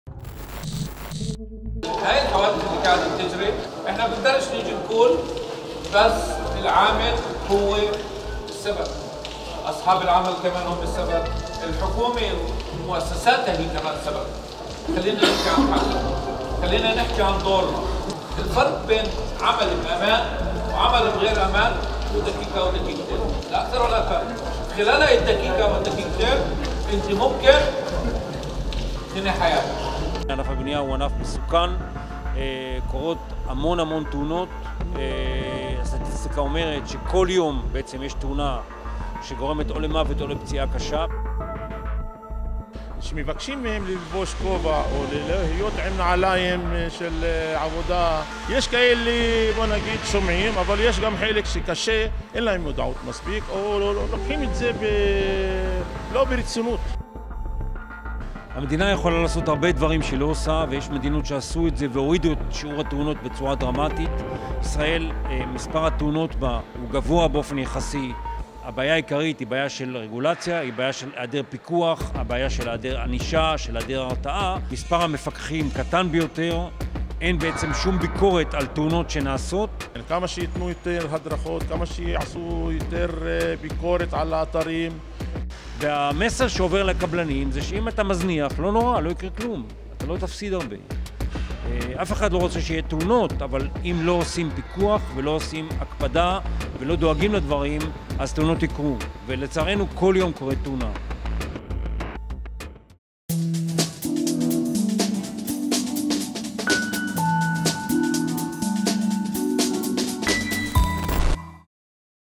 כל יום קורית תאונה
הסטטיסטיקה אומרת שכל יום קורית תאונת עבודה בענף הבניין, שסופה הוא מוות או פציעה קשה. במסגרת פעילות של ארגון העובדים מען, נפגש מהנדס הבטיחות מוחמד ג'יוסי עם פועלים באתר בניה בחולון לשיחה על בטיחות ותאונות עבודה.